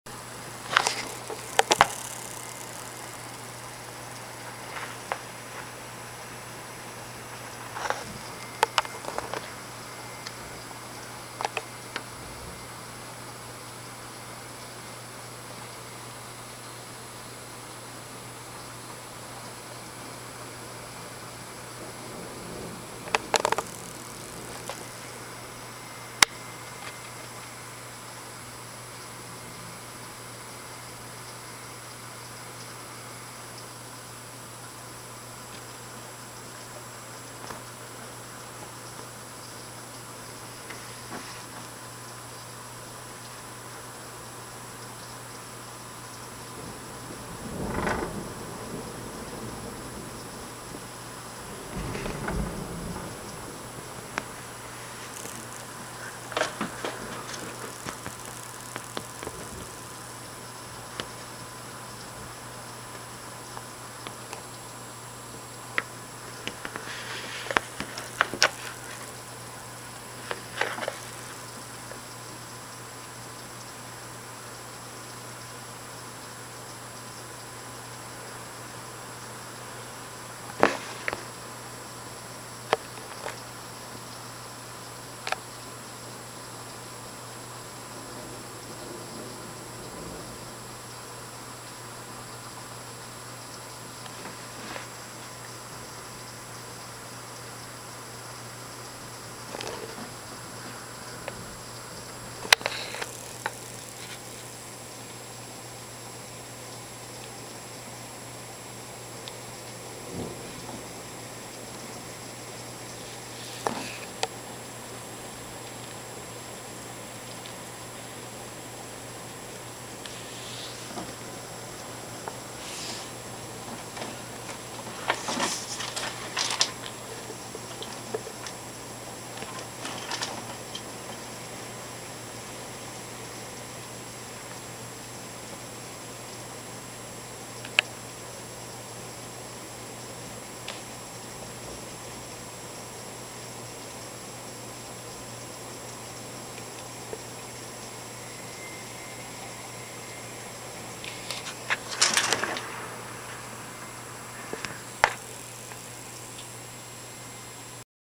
Heavy rain during a thunderstorm/downburst in Maryland
(recorded May 11, 2013) This storm caused a downburst about 10 miles east of my house.